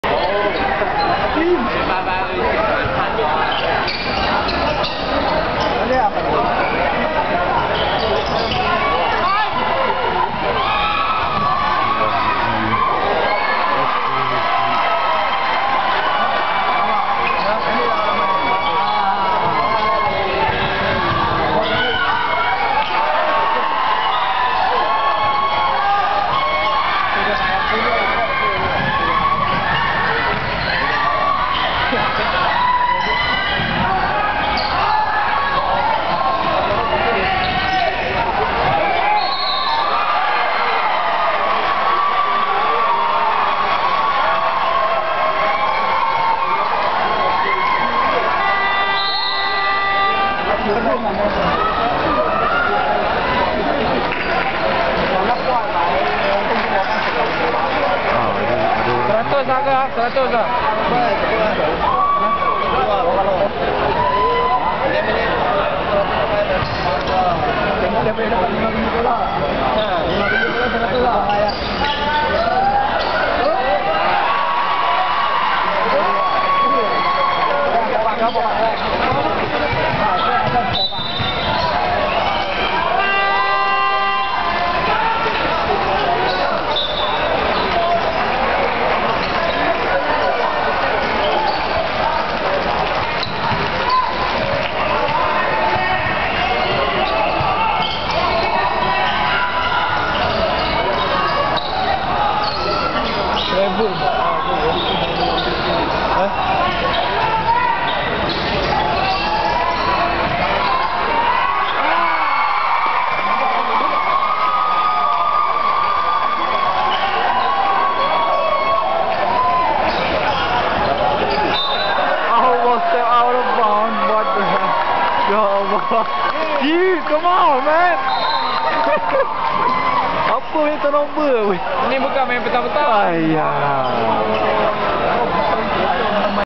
SEABA Junior Championship at MABA Stadium 1 May 2008
Malaysia vs Philippines SEABA Junior Championship at MABA Stadium 1 - 4 May 2008.

Malaysians were leading by 4 or so points during the first half but were outplayed by the philippines full court press in the 3rd quarter. Malaysia only managed to score 4 points on that quarter :(

Nice jerseys tho ;)

edit: i think someone was using a phone when i took the vid